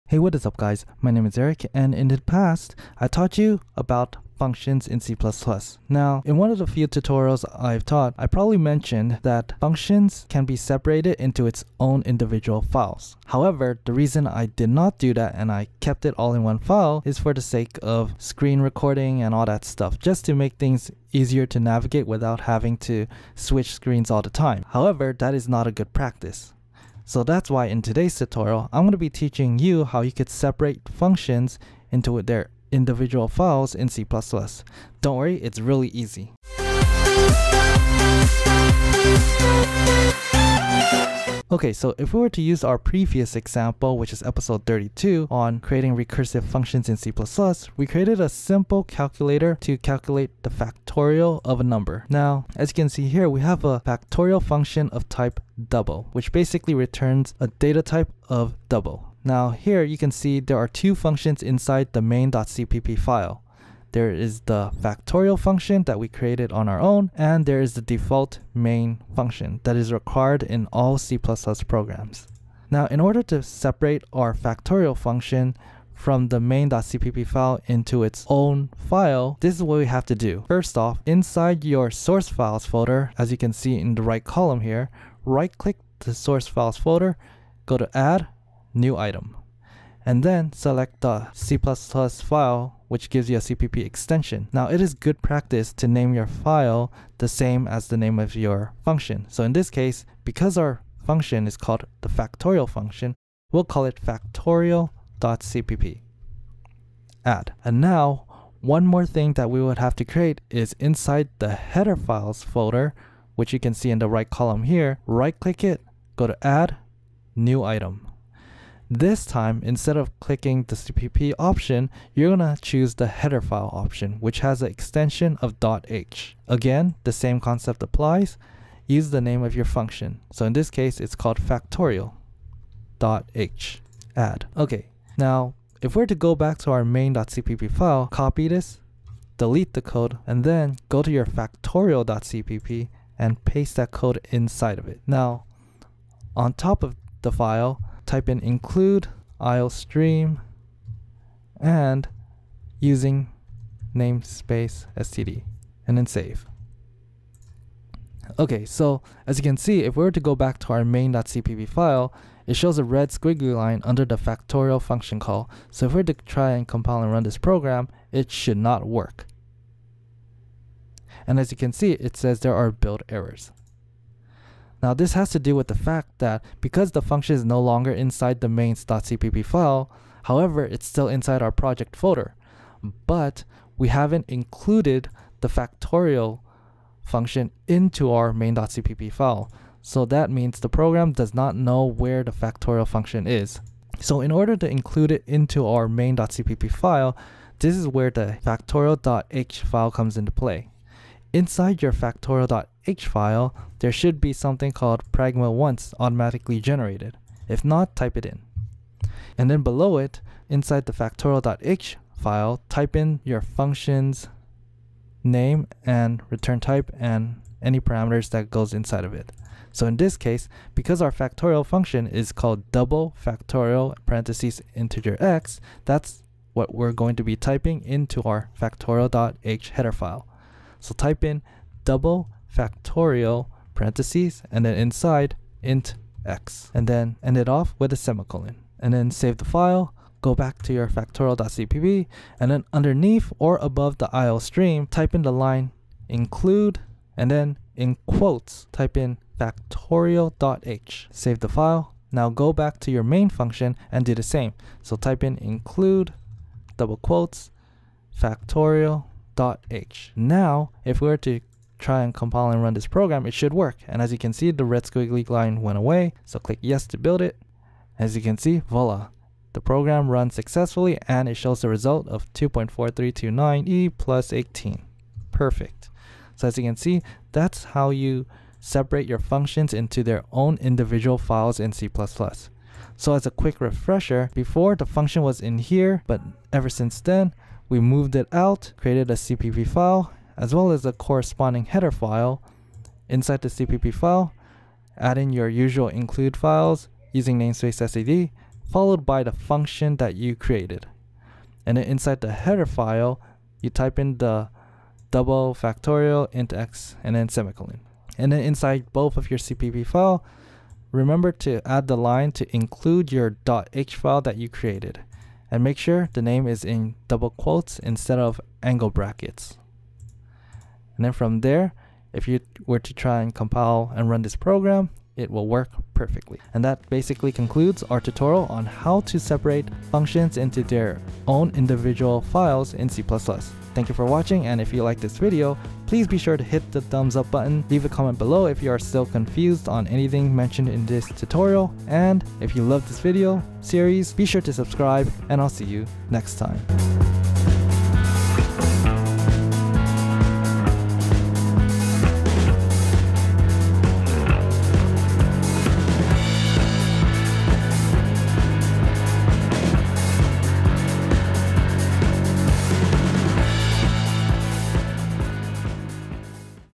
C++ Programming Tutorials - 33 - C++ Functions In Separate Files - Eric Liang
Learn how to create functions in C++ in their individual files. Back when I covered how to create functions in C++, I mentioned that we can create functions in their own files, but decided to not do that because it would make the tutorials harder to follow if I had to keep on switching files every time. So, in this tutorial, we will finally cover the topic of how to separate them because it's good practice to do so especially when your program gets bigger. It also makes maintaining code a lot more easier.

The introduction music used in this video is created by Tobu and Itro.
I do not take any credits for the creation of the music content.